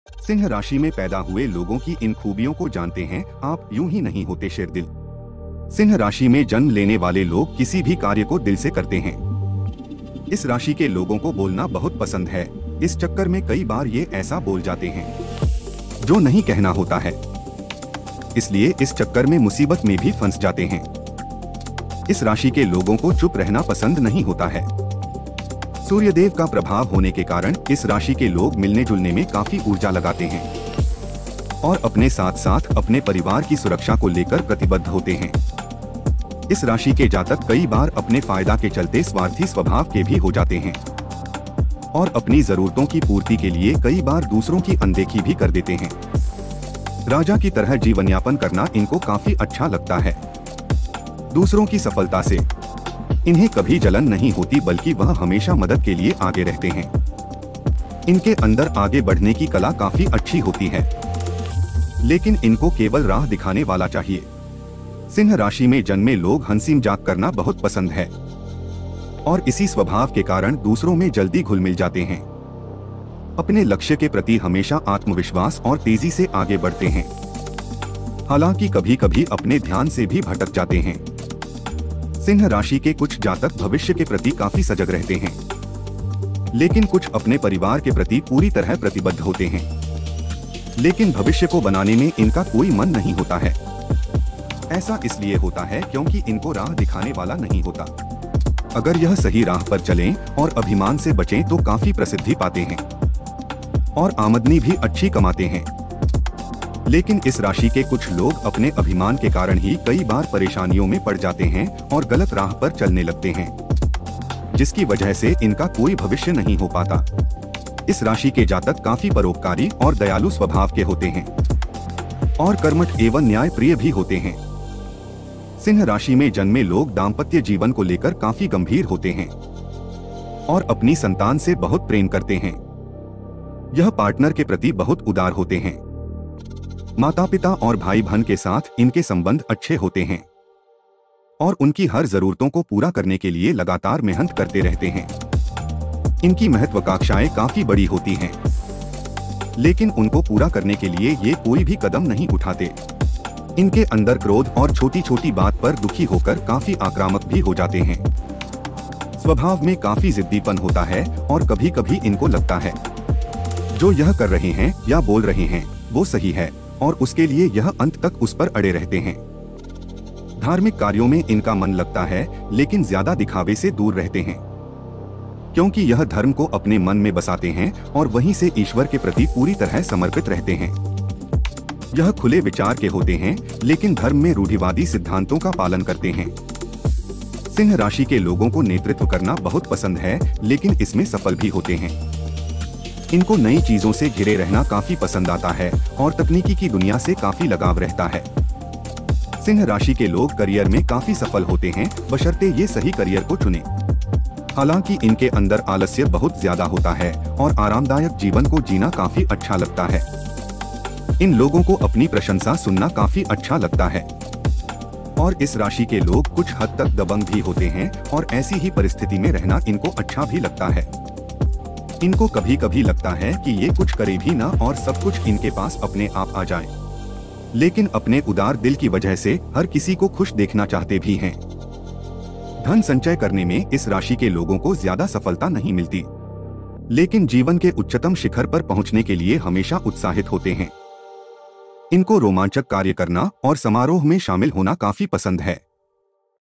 सिंह राशि में पैदा हुए लोगों की इन खूबियों को जानते हैं आप यूं ही नहीं होते शेरदिल leo | Holy Vlog
सिंह राशि में पैदा हुए लोगों की इन खूबियों को जानते हैं आप यूं ही नहीं होते शेरदिल leo | Holy VlogWelcome to the Holy Vlog